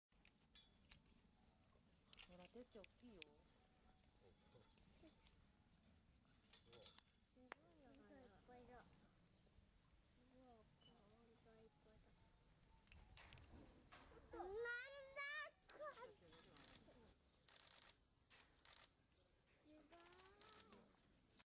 フェンスに登ってるユキヒョウっ子 Snow leopard cub climbing a fence.
彼は、昨年の5月25日に多摩動物公園で生まれた三つ子の「皇海(スカイ)」です。
彼は、フェンスに登ってたです。
彼は、1才になって、大きくなって、やんちゃな子になったです。
He is "Sukai" who is triplet and was born May 25th of last year in "Tama Zoological Park".
He was climbing a fence.
He is 1 year old and became bigger, and mischievous kid.